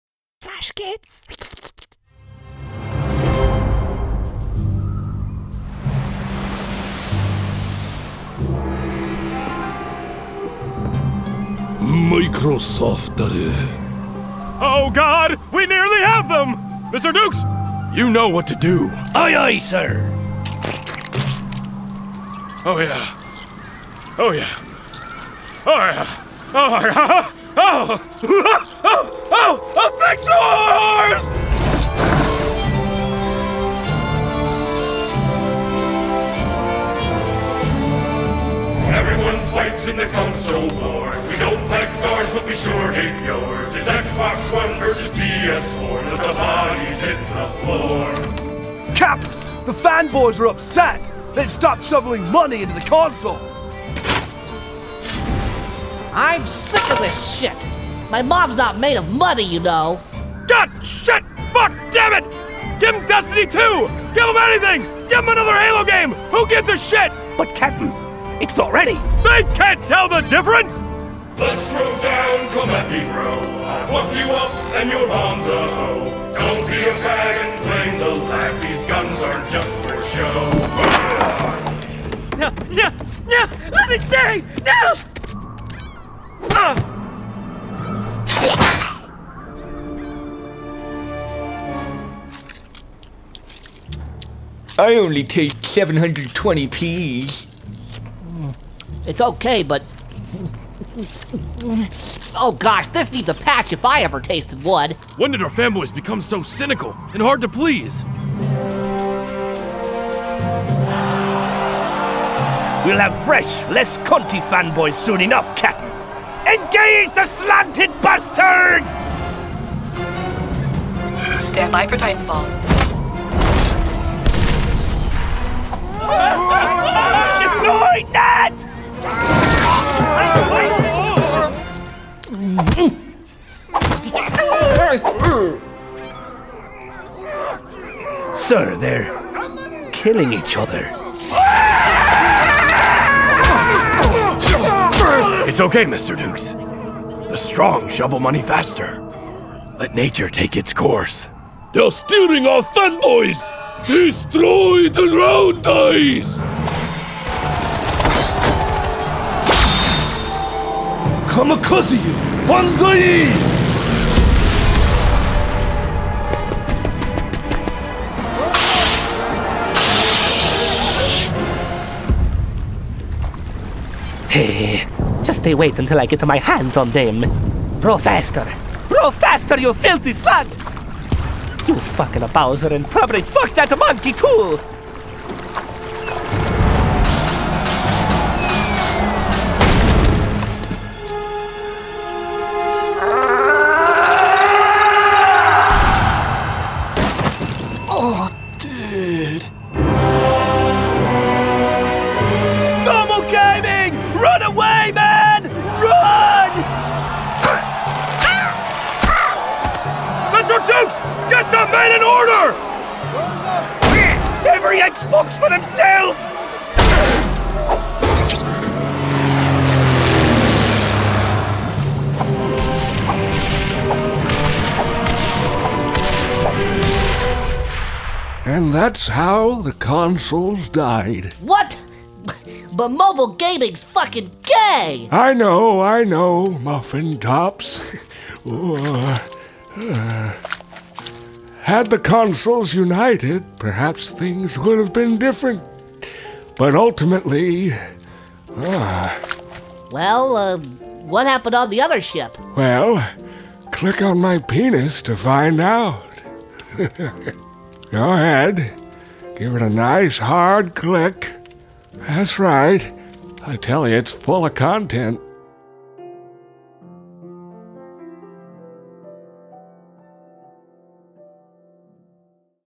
Console Wars - XBOX ONE
It's XBOX ONE vs PS4, blood will be spilt! But who will be victorious?

Created by Tom Hinchliffe and Don Greger

Huge thanks to James Lee for additional animation!

Voice-Over:
Alex Walker Smith
Mike Winters

Sound by: Mike Winters